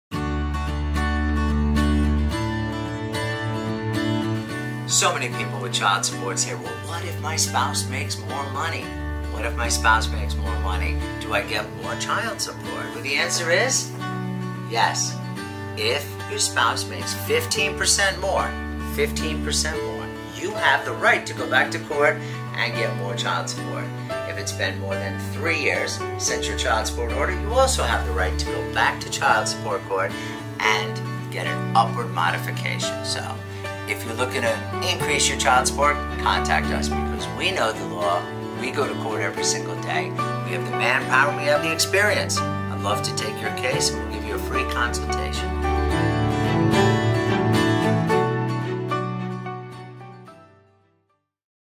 Raising Child Support Payments | Bryan L. Salamone & Associates, P.C.
So many people who receive child support payments ask, "What if my spouse makes more money? Can I get more child support?"

Contract Bryan L. Salamone & Associates, P.C. for assistant in filing an upward modification of child support